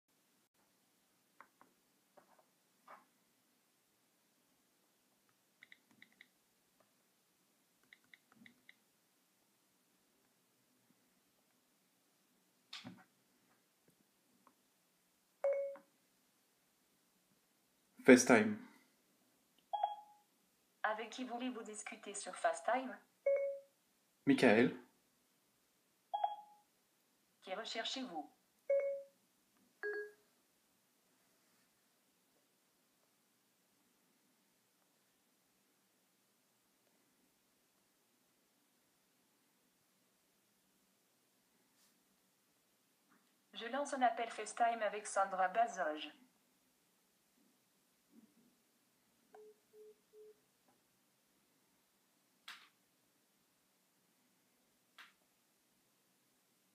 Faille de Siri sous iOS 7.1.1
Cette faille de Siri sur un iPad verrouillé sous iOS 7.1.1 permet d'accéder au carnet d'adresses.